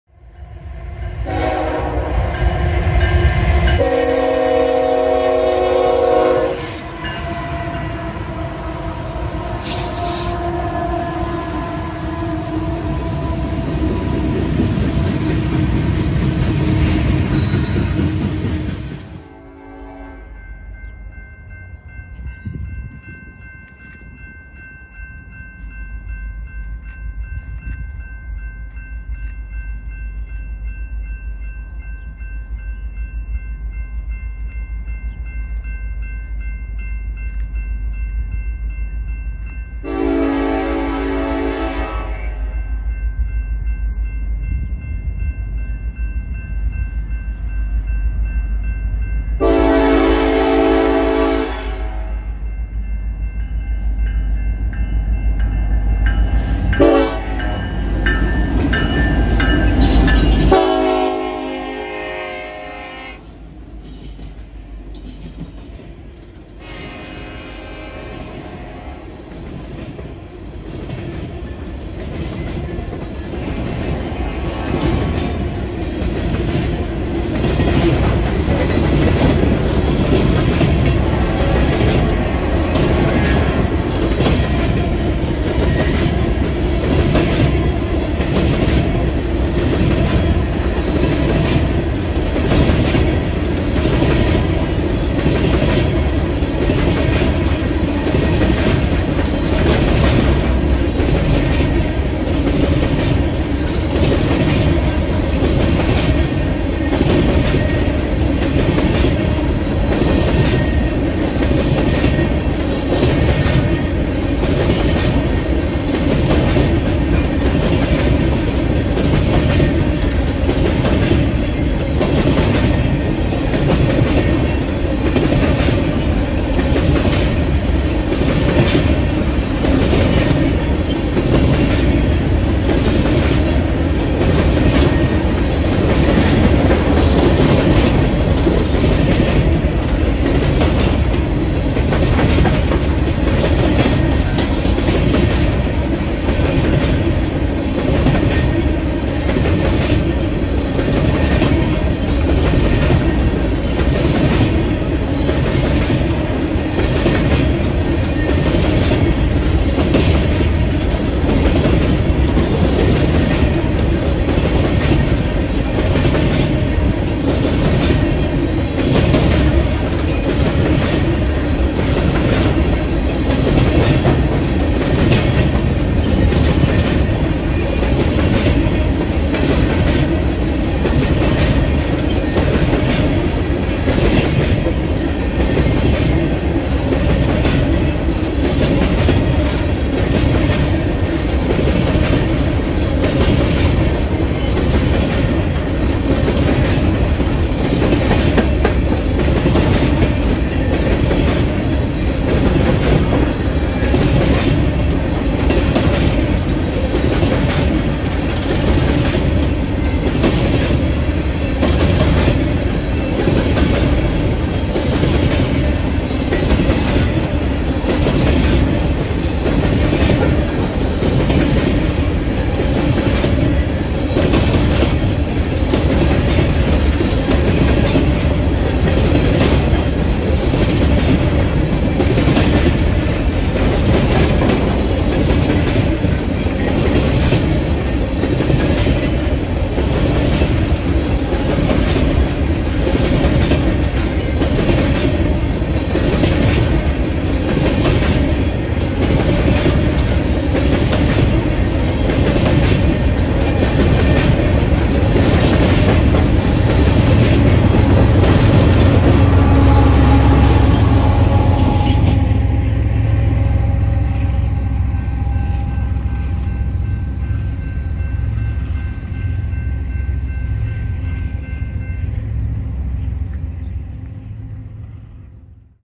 E CECSCM With a Nice ACe Leader
Today I went to the Benton Crossing to get an EB empty coal train

Chapters:
0:00 Intro
0:19 E CECSCM

*Impersonation or reposting this channel and any content from it will result in consequences.*